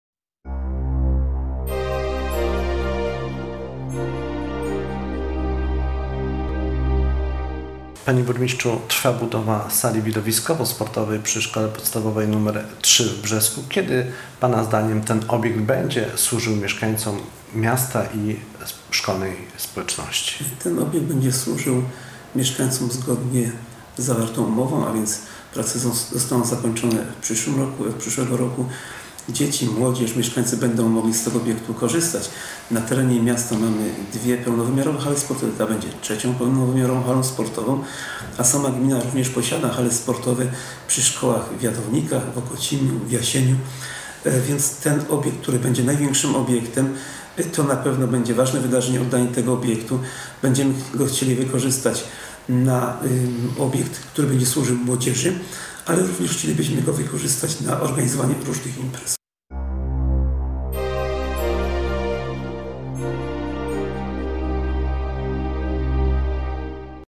Pełnowymiarowa hala przy PSP 3
W przyszłym roku zakończy się budowa hali przy Publicznej Szkole Podstawowej nr 3 w Brzesku.